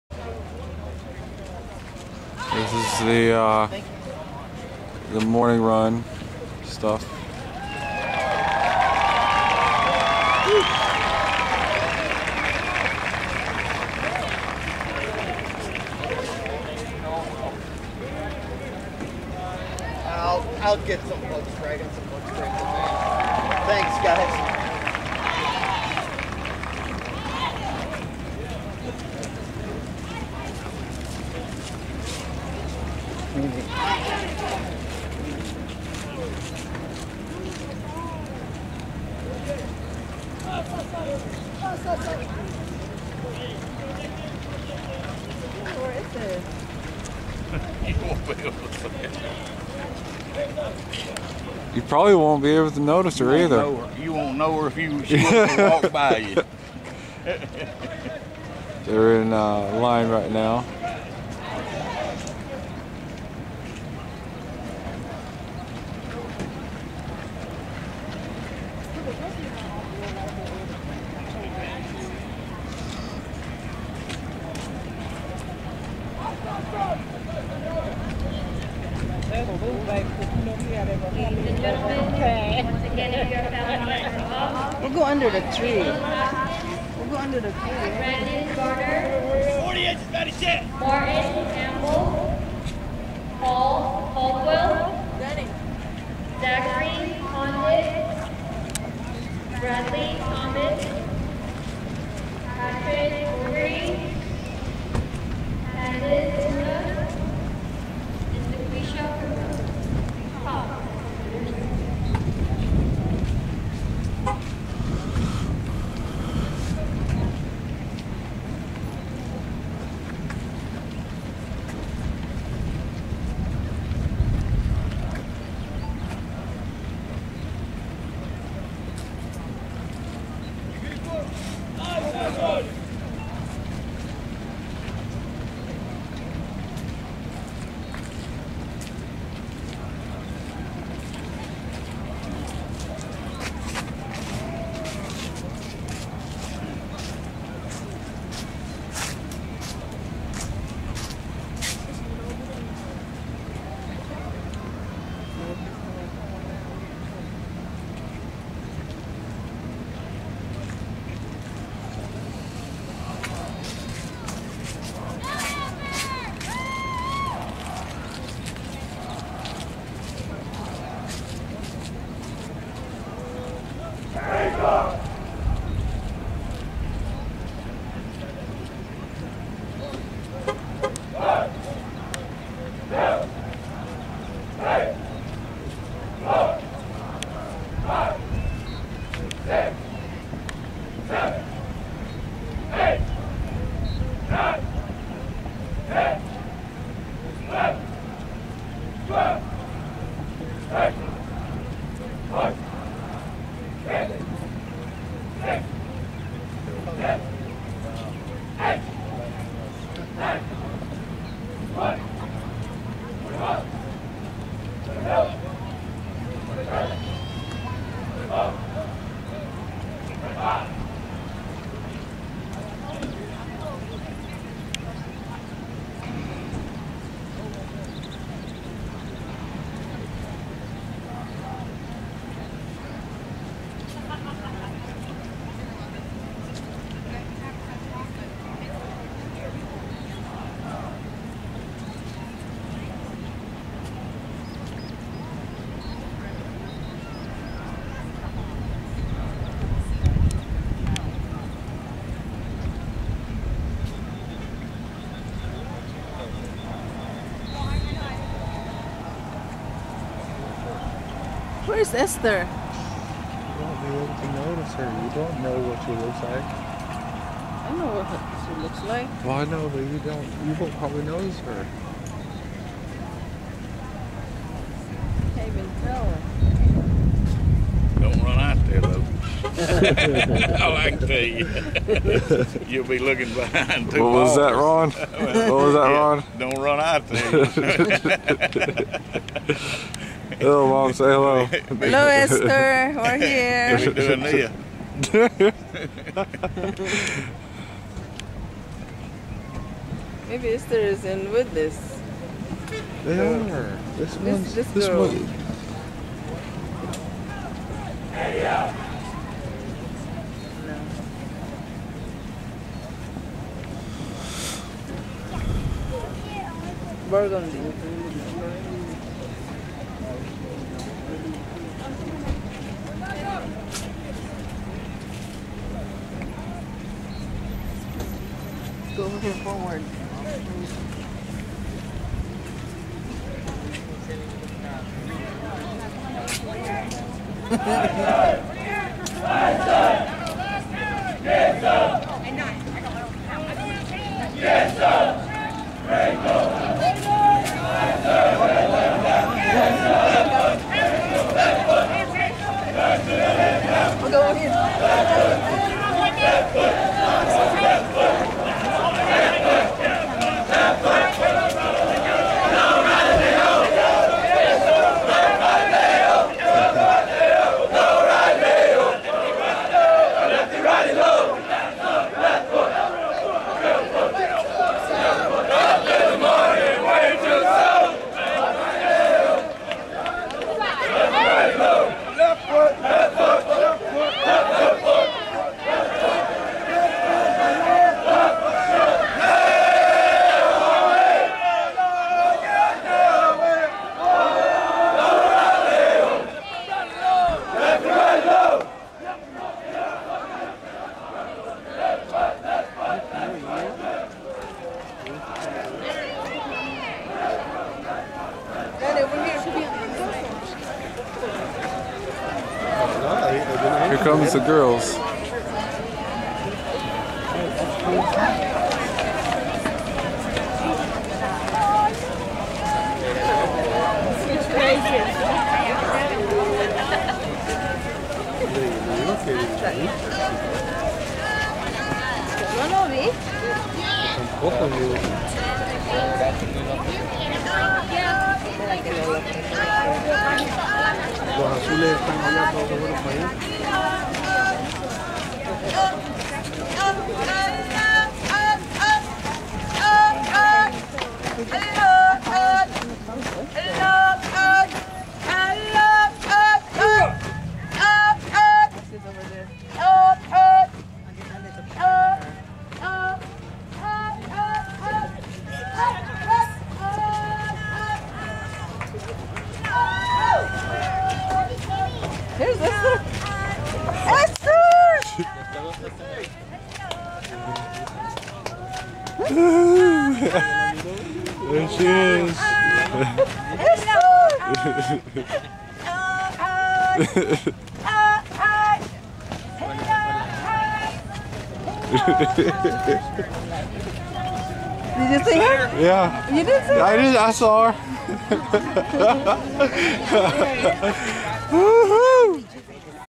pt 14 my daughter graduates from parris island south carolina. my ex wife finally sees her
my daughter graduates from parris island